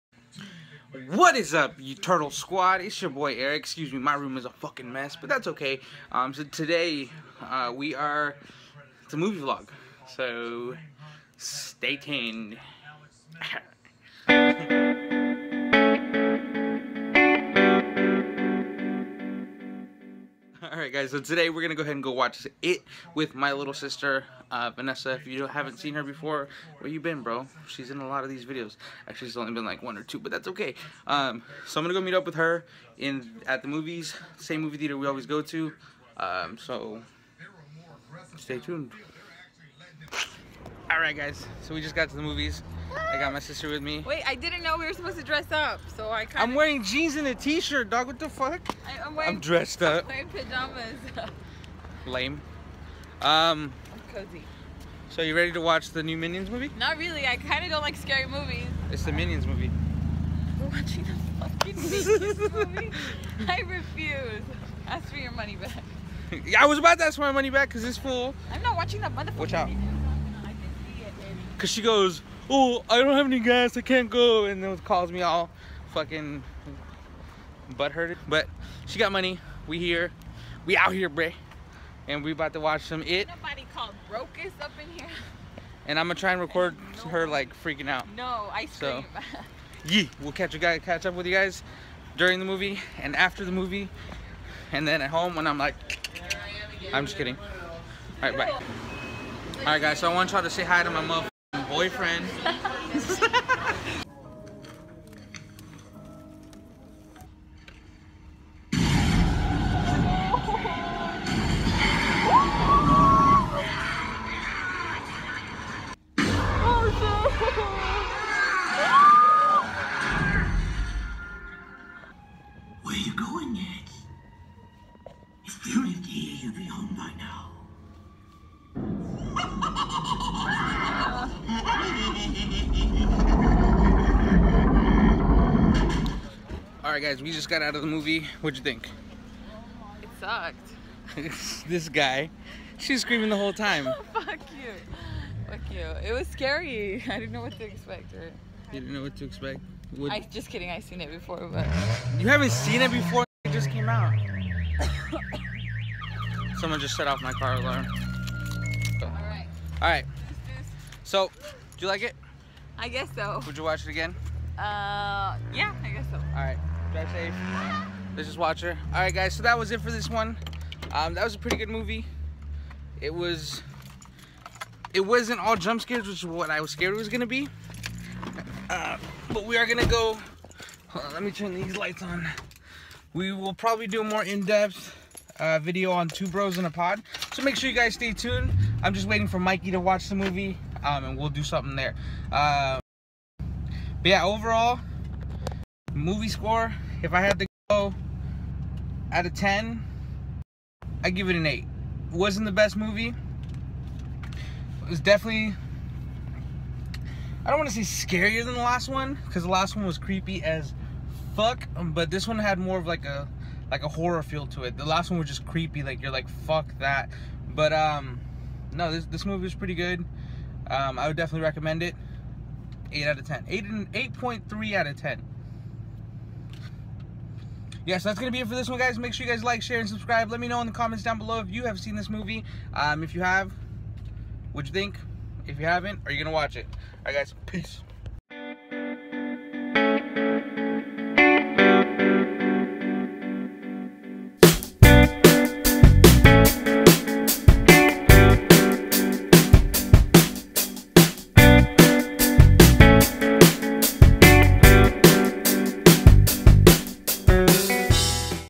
Mini Movie Vlog: IT // Spoiler Free // Hanging out with my Sis!
Whats up Turtle Squad!

IT finally hit theaters and we couldn't wait to go watch it ! Not a crazy long blog but we thought we would bring you along with us regardless and hope you enjoy!

Add me on PSN: AliveEKG

Intro by Danny Minick, show him some love!